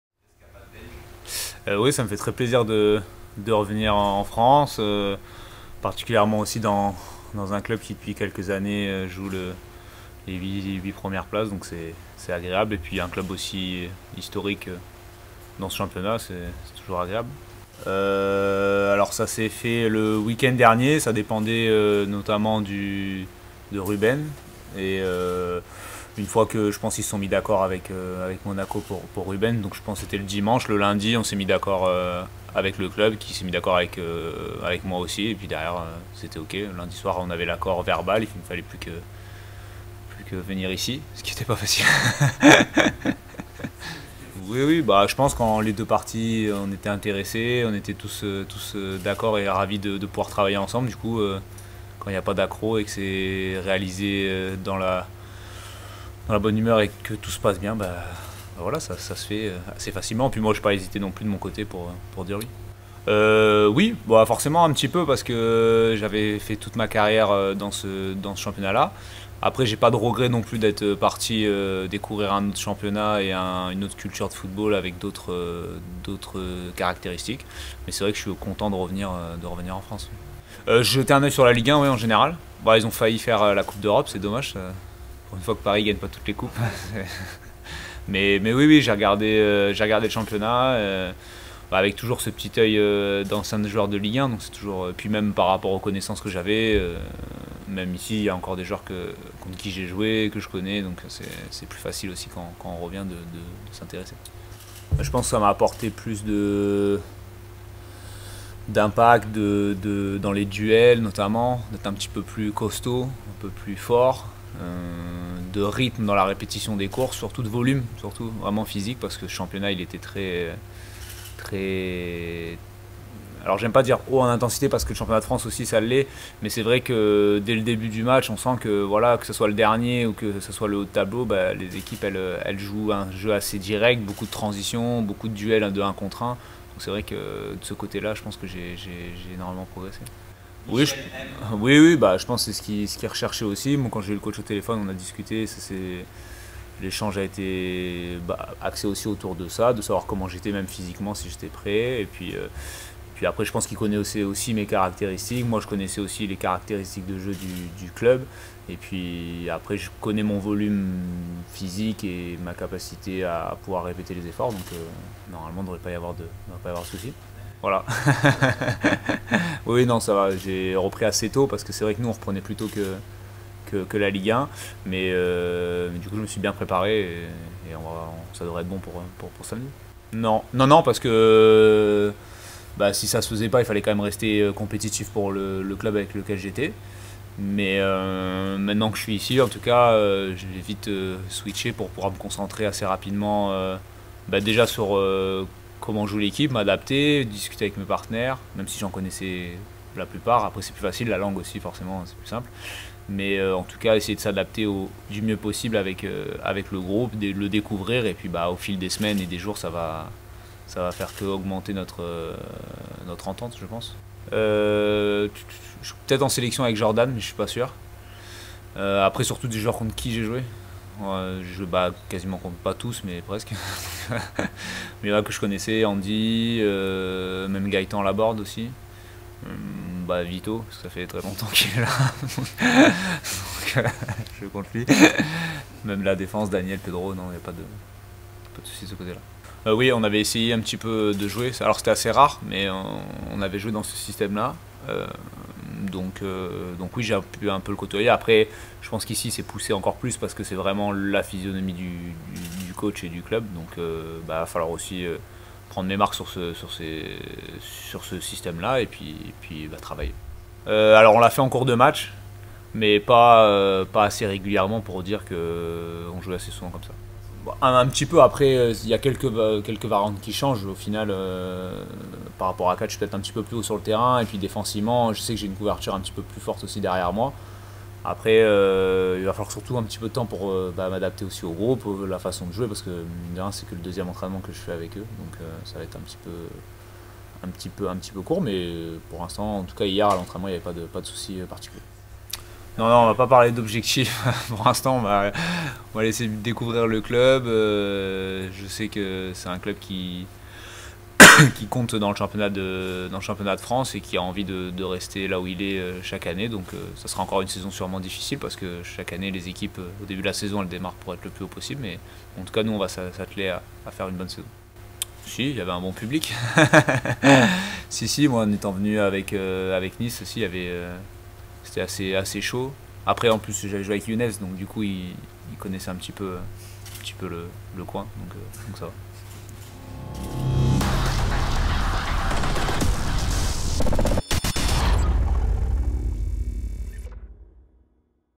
Arnaud Souquet avant MHSC vs SRFC
J-2 avant la réception du Stade Rennais à La Mosson, écoutez le nouveau défenseur du Montpellier Hérault Sport Club (1ère journée Ligue 1 Conforama).